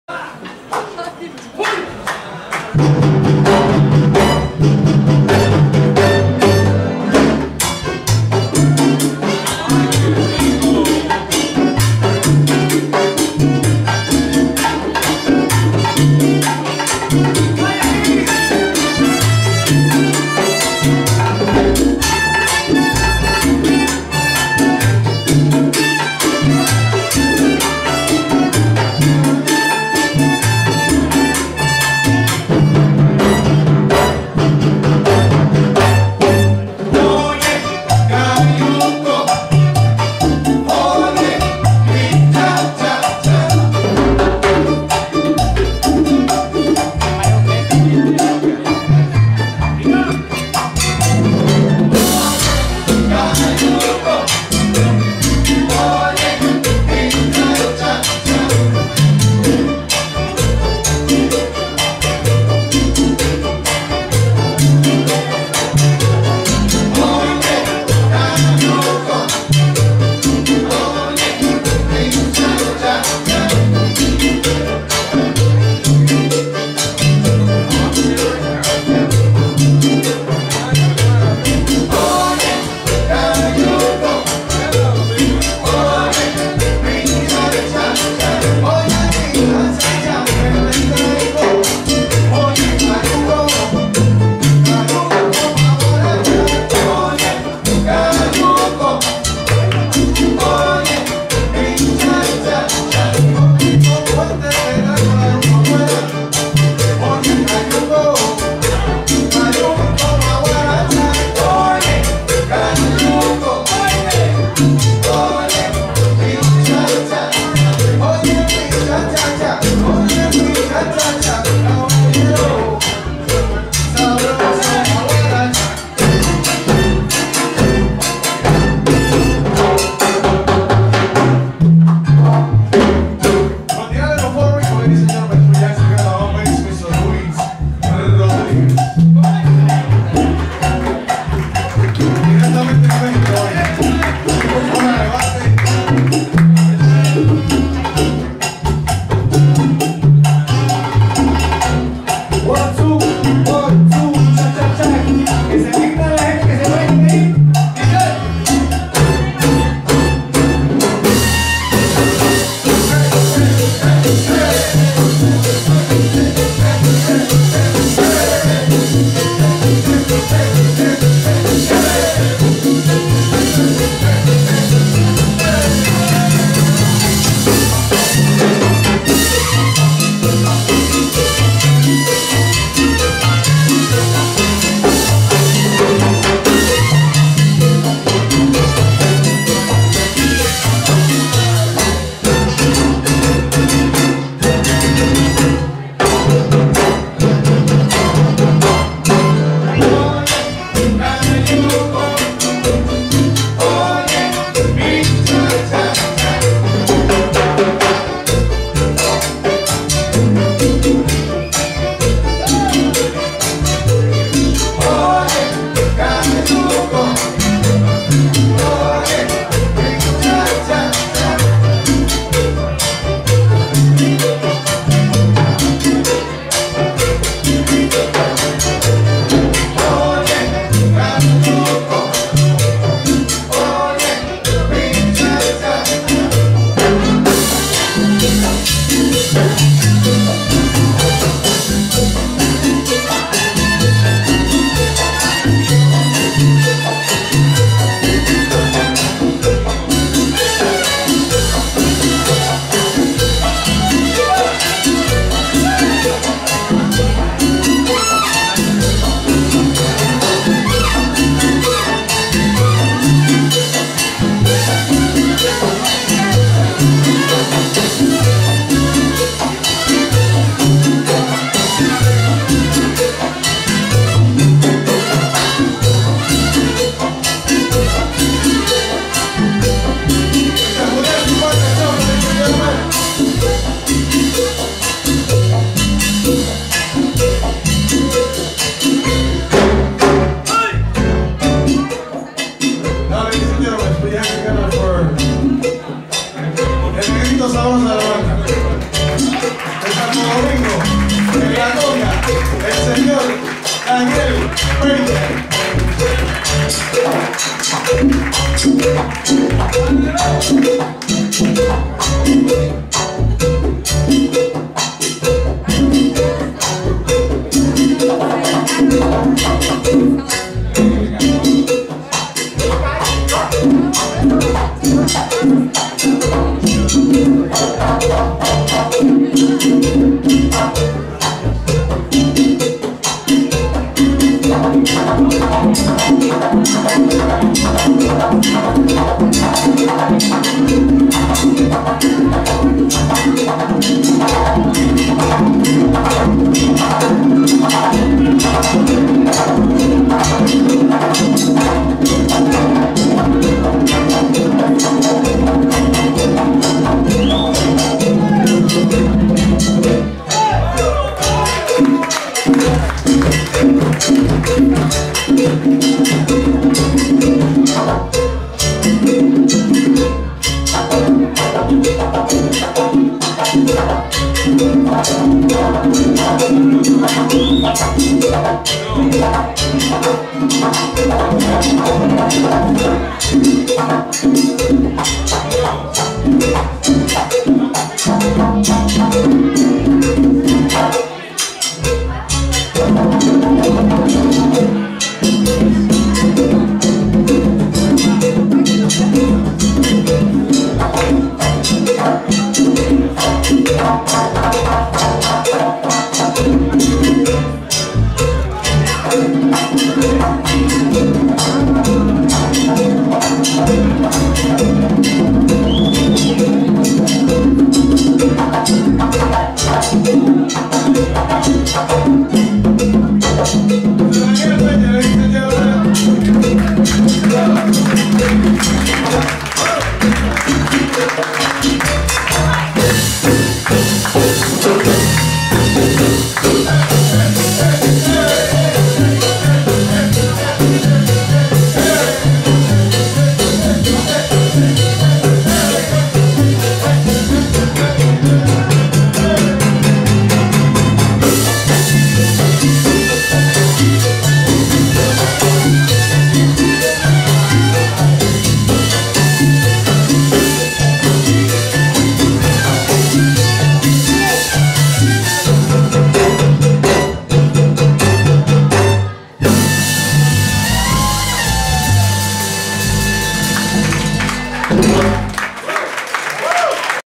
KCC Productions presents Tito Puente, Jr.'s, Latin Jazz Ensemble
At the Van Dyke Cafe, Dec 18, 2010. Toy drive and concert for Miami Children's Hospital Foundation.